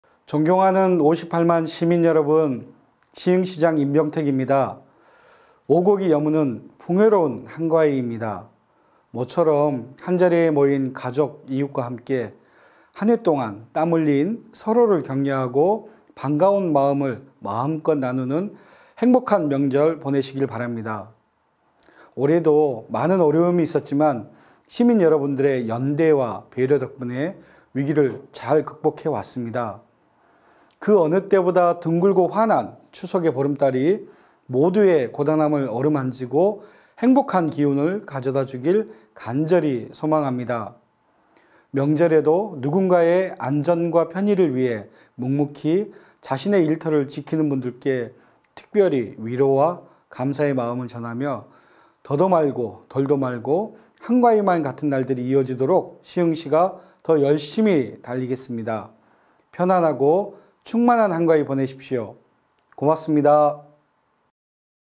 [시흥시장 임병택] 넉넉하고 풍요로운 추석 보내세요
존경하는 58만 시민 여러분,
시흥시장 임병택입니다.

오곡이 여무는 풍요로운 한가위입니다.
모처럼 한자리에 모인 가족, 이웃과 함께
한 해 동안 땀 흘린 서로를 격려하고,
반가운 마음을 마음껏 나누는
행복한 명절 보내시길 바랍니다.

올해도 많은 어려움이 있었지만,
시민 여러분의 연대와 배려 덕분에 위기를 잘 극복해 왔습니다.

그 어느 때보다 둥글고 환한 추석의 보름달이
모두의 고단함을 어루만지고,
행복한 기운을 가져다주길 간절히 소망합니다.

명절에도 누군가의 안전과 편의를 위해
묵묵히 자신의 일터를 지키는 분들께
특별히 위로와 감사의 마음을 전하며
더도 말고 덜도 말고 한가위만 같은 날들이 이어지도록
시흥시가 더 열심히 달리겠습니다.

편안하고 충만한 한가위 보내십시오.
고맙습니다.